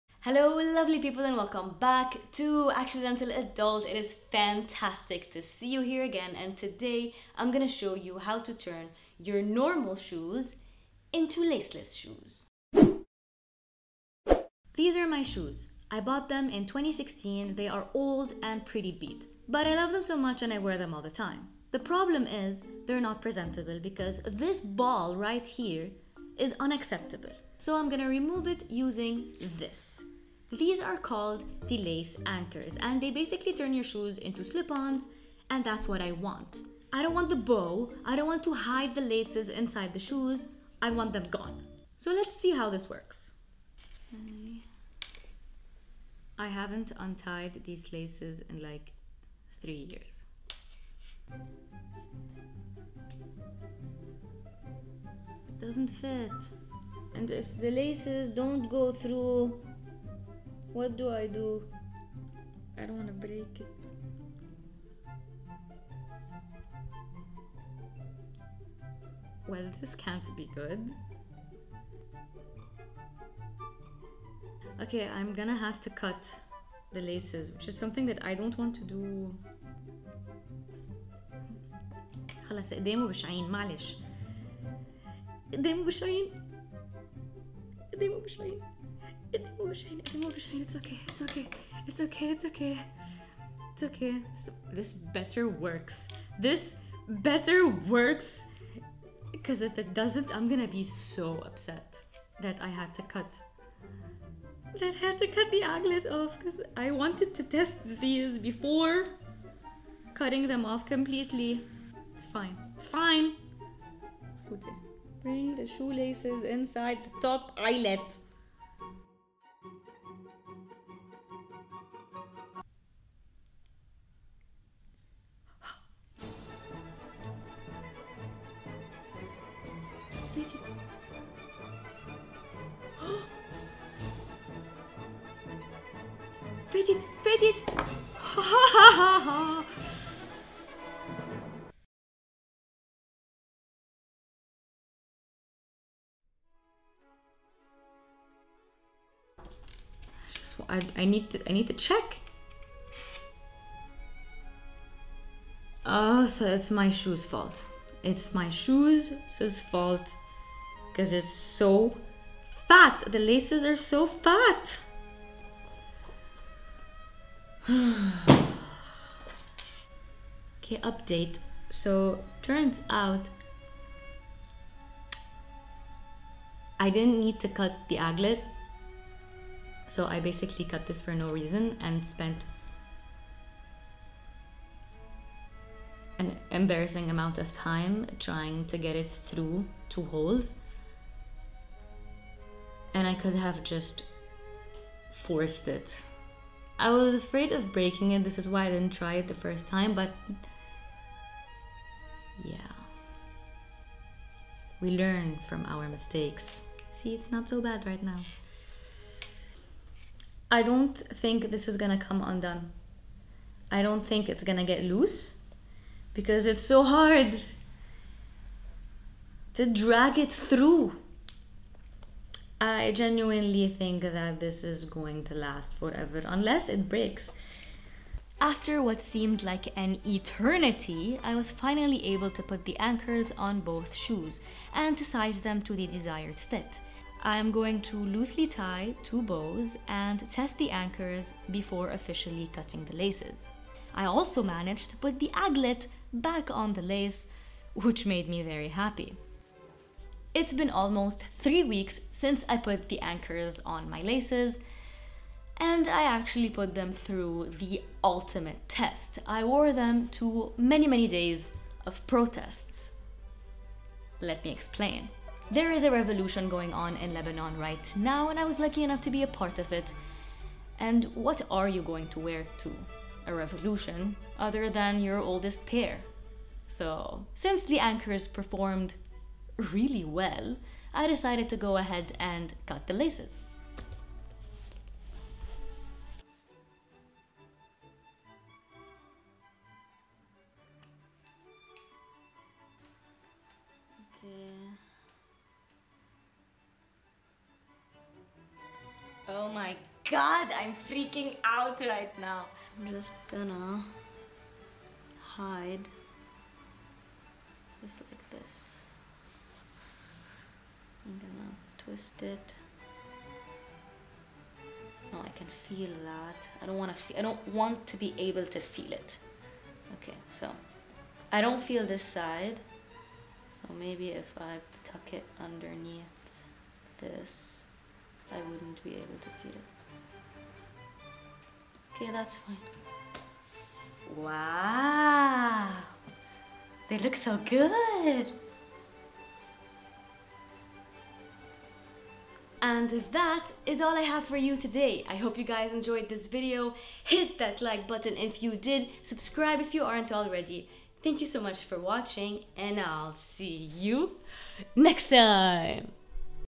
How to Turn Your Shoes Into Laceless Shoes
I'm not a fan of a bow on my shoes. I prefer a clean look without laces. So instead of hiding them inside the shoes, I wanted to see if I can remove the laces completely without sacrificing grip. Turns out you can!